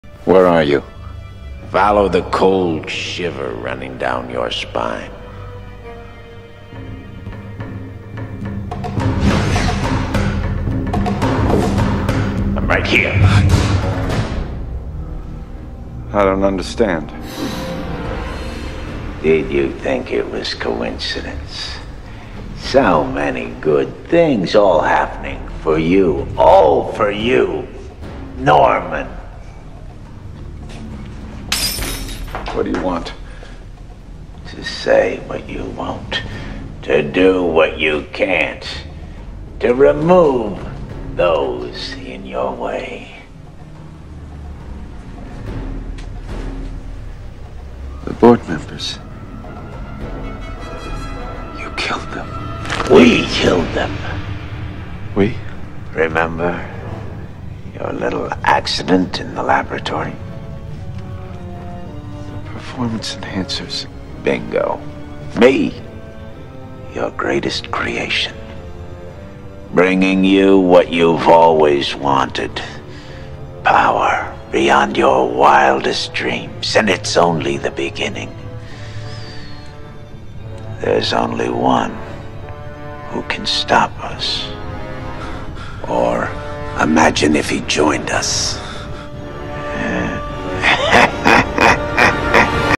Green Goblin and Norman same body two minds
They may be the same person but they have a truly different personality.